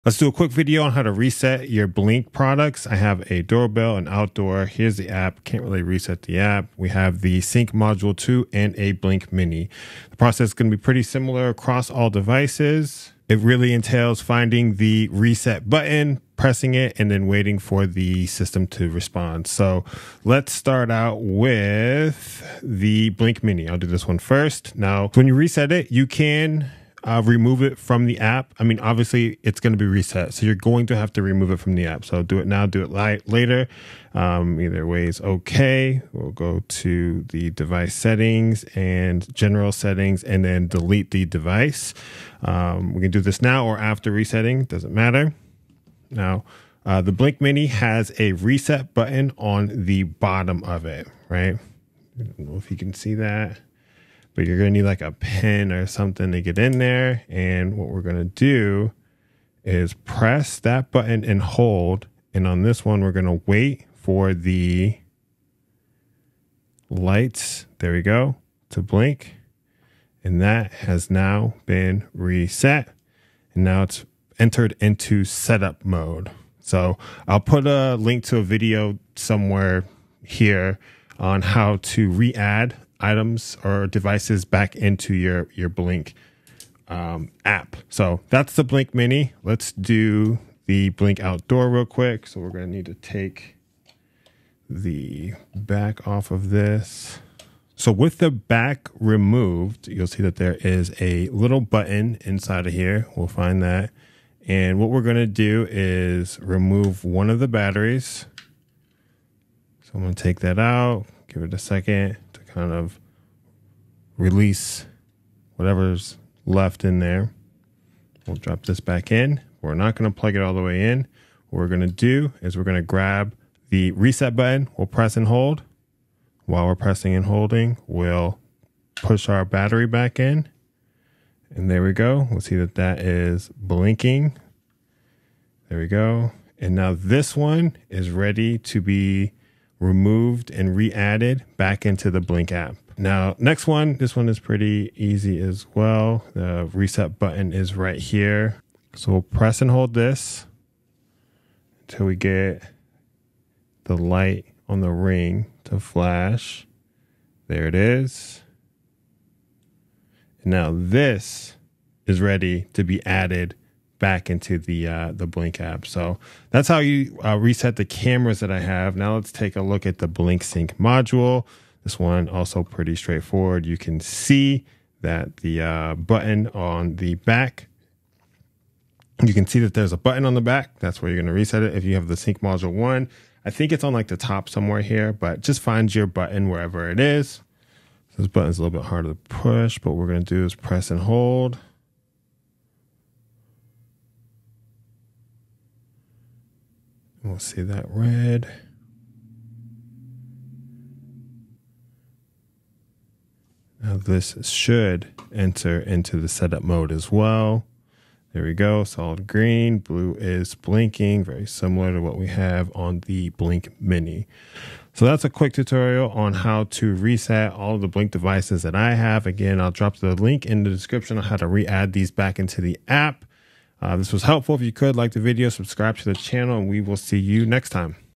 Reset Blink Cameras (Outdoor, Mini, Doorbell & Sync Module)
Here is a quick video on how to reset your Blink devices.

Products seen in video:
Blink Outdoor Cam

Blink Video Doorbell

Blink Mini

Blink Sync Module

Main Facebook Page:
 / smarthomestarter
 / smarthomestarter
 / smarthomestarte

00:00 Introduction
00:30 Remove device from app
00:45 Reset Blink Mini
01:29 Reset Blink Outdoor
02:23 Reset Blink Doorbell
02:48 Reset Blink Sync Module